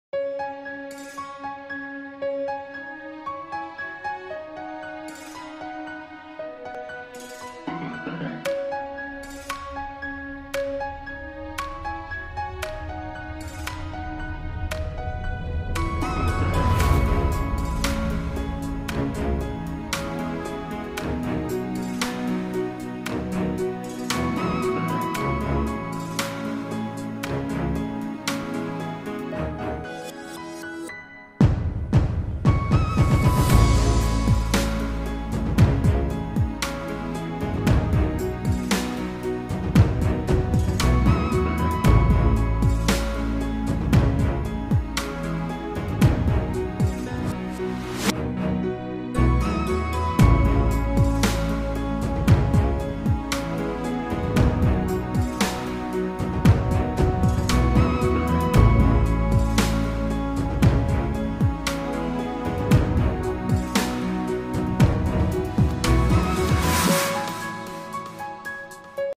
Kids Tent House || Play House || Foldable
Playing Tent House for kids.
It is easy to set up and easy toh fold down.